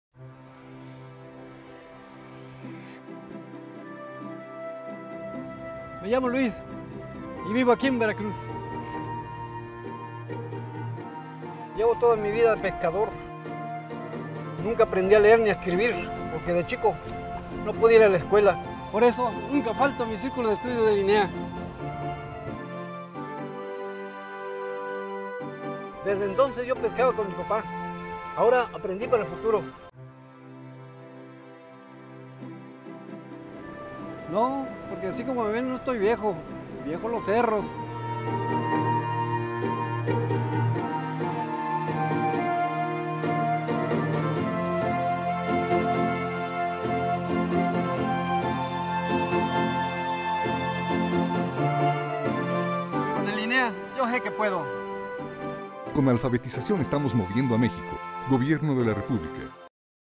INEA CINEMINUTO VERACRUZ
Cineminuto para la campaña "Yo sé que puedo" para la Instituto Nacional para la Educación de los Adultos (INEA)
Director: Gerardo Pardo
Fotógrafo: Alberto "Chino" Lee